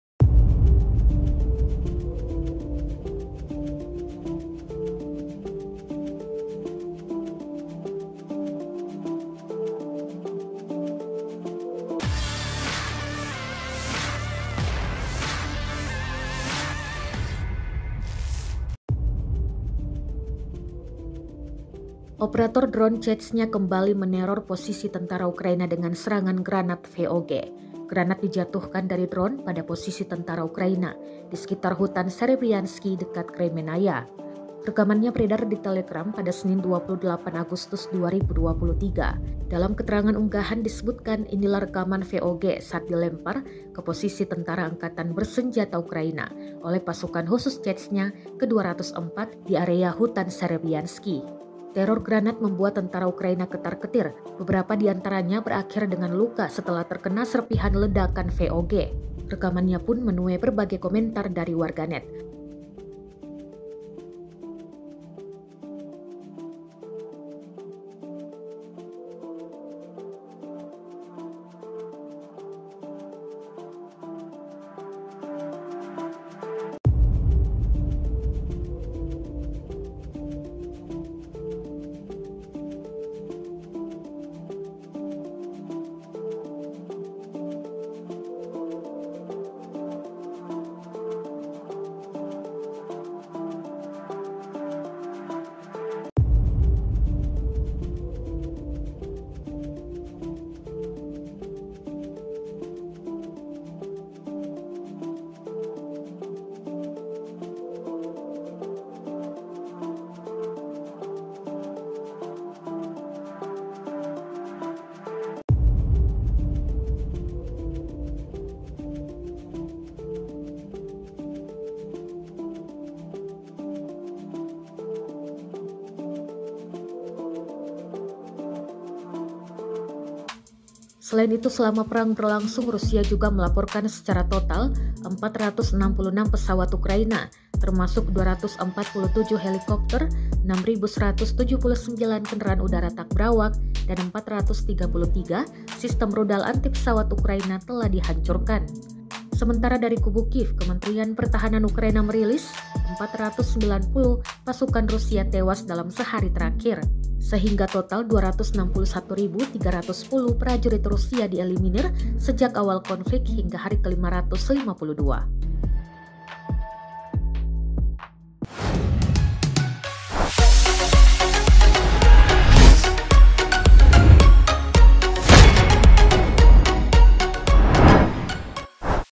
Teror Granat VOG Chechnya Bikin Tentara Ukraina Ketar-ketir
TRIBUN-TIMUR.COM - Operator drone Chechnya kembali meneror posisi Tentara Ukraina dengan serangan granat VOG.

Granat dijatuhkan dari drone pada posisi Tentara Ukraina di sekitar hutan Serebryansky dekat Kremennaya.

Rekamannya beredar di Telegam pada Senin, 28 Agustus 2023.

Dalam keterangan unggahan disebutkan inilah rekaman VOG saat dilempar ke posisi militan Angkatan Bersenjata Ukraina oleh Pasukan Khusus Chechnya ke-204 di area hutan Serebryansky.

Teror granat membuat Tentara Ukraina ketar-ketir.

Beberapa di antaranya berakhir dengan luka setelah terkena serpihan ledakan VOG.

Rekamannyapun menuai berbagai komentar dari warganet.

Selain itu selama perang berlangsung Rusia juga melaporkan secara total, 466 pesawat Ukraina, termasuk 247 helikopter, 6.179 kendaraan udara tak berawak dan 433 sistem rudal anti-pesawat Ukraina telah dihancurkan.

Sementara dari kubu Kyiv, kementerian pertahanan Ukraina merilis 490 Pasukan Rusia tewas dalam sehari terakhir.

Sehingga total 261.310 Prajurit Rusia dieliminir sejak awal konflik hingga hari ke-552.

Narator: Ririn